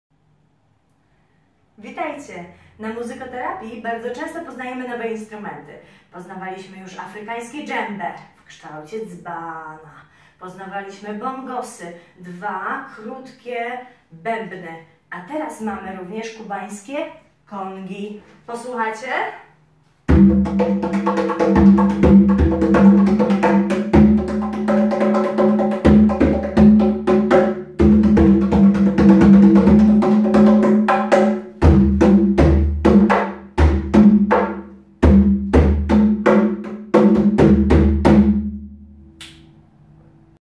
Muzykoterapeutycznie z przedszkolakami - Poznajemy instrumenty: KONGI (CONGAS)
Kongi to instrumenty kubańskie wywodzące się z Afryki. Są to instrumenty, których korpus jest projektowany na podobieństwo beczek - poprzez łączenie drewnianych listew (w odróżnieniu od afrykańskich bębnów drążonych z pni drzew). Naciągi, w które uderzamy dłońmi lub drewnianymi pałeczkami, są zrobione z naturalnej lub syntetycznej skóry przymocowanej metalową obręczą.